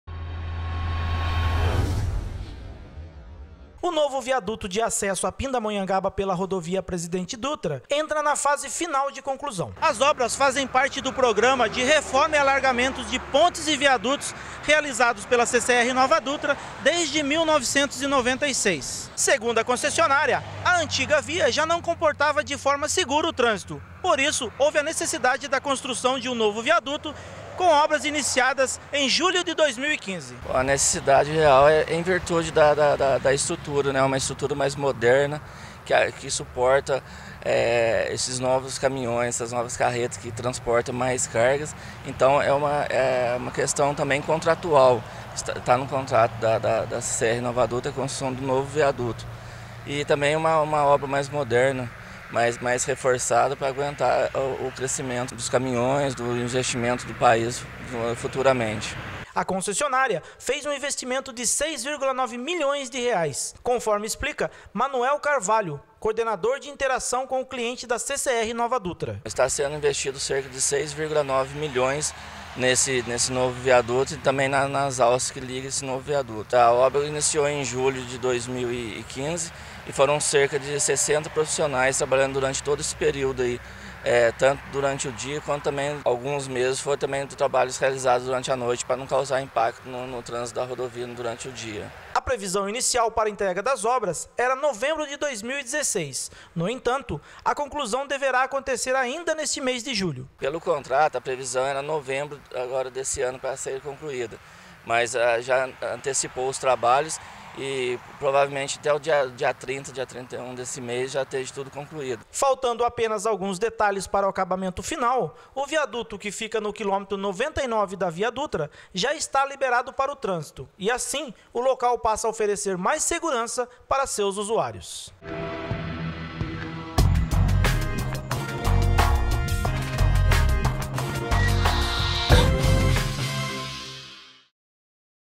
Novo viaduto oferece mais segurança no acesso à Pinda pela via Dutra
O novo viaduto de acesso à Pindamonhangaba pela rodovia Presidente Dutra entra na fase final de conclusão.

 A concessionária fez um investimento de R$ 6,9 milhões de reais, conforme explica Manoel Carvalho, Coordenador de Interação com o Cliente da CCR NovaDutra.

 A previsão inicial para a entrega das obras era novembro de 2016, no entanto, a conclusão deverá acontecer ainda neste mês de julho.